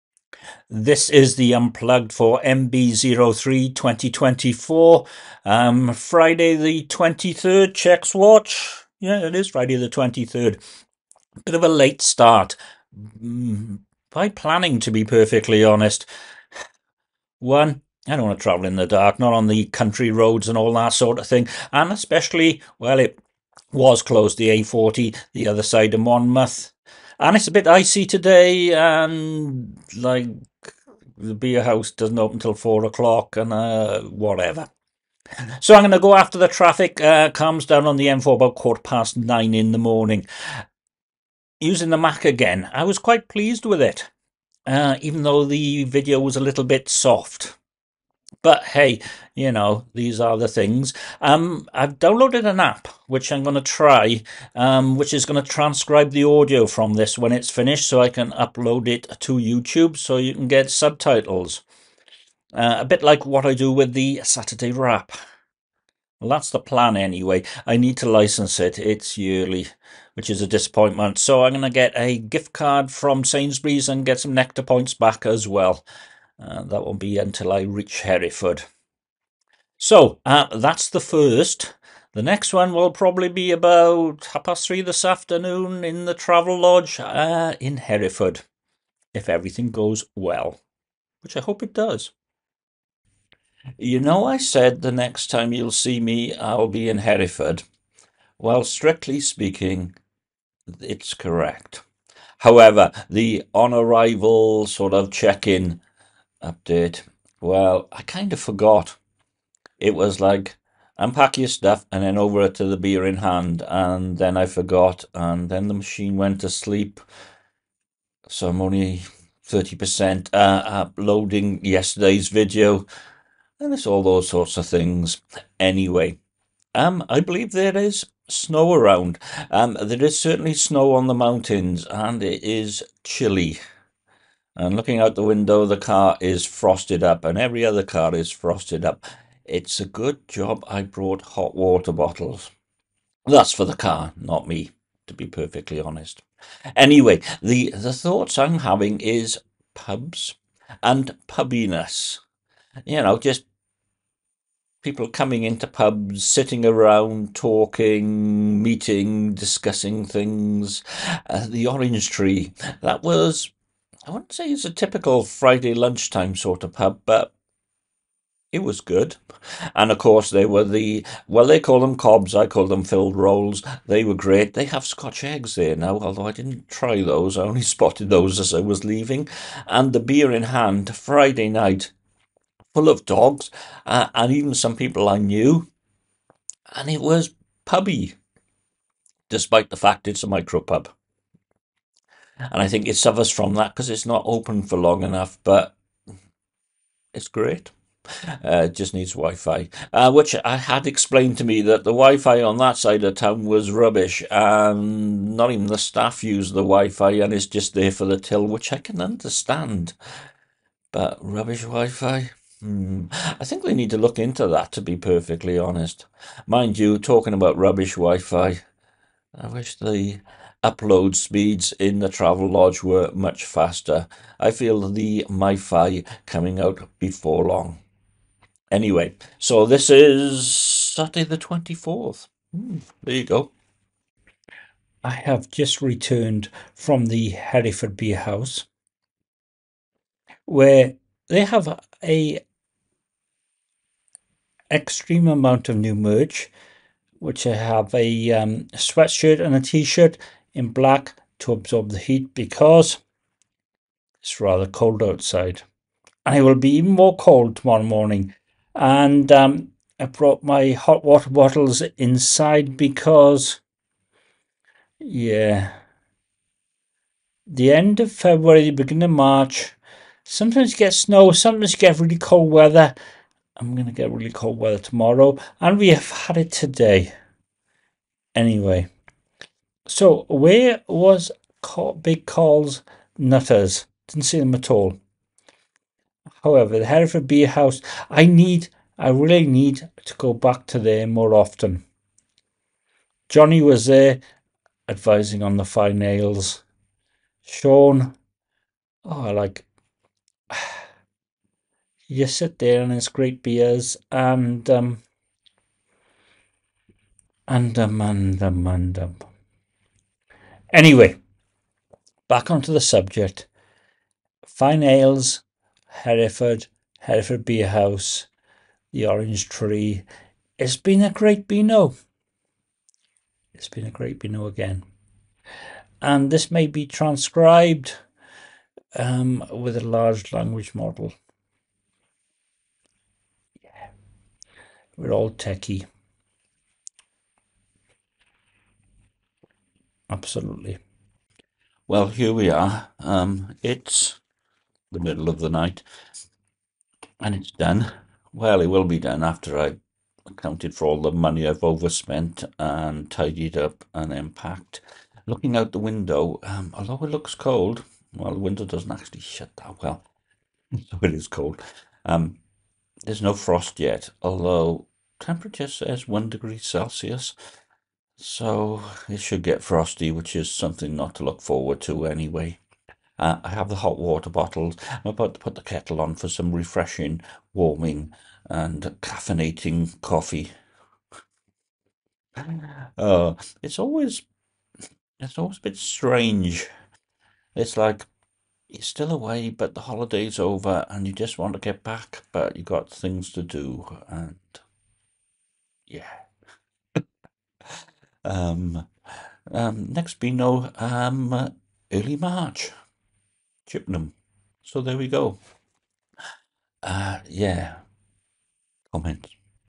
mb03 2024 Unplugged
Well, the change of hardware worked even if the result is slight soft. However, this time I will be able to generate subtitles.

The on check in video is missing, if for no other reason than I forgot. Updates on the weather and The Orange Tree and Beer in Hand.

Post Hereford Beer House Saturday afternoon and pre departure from Hereford. A Great Beano.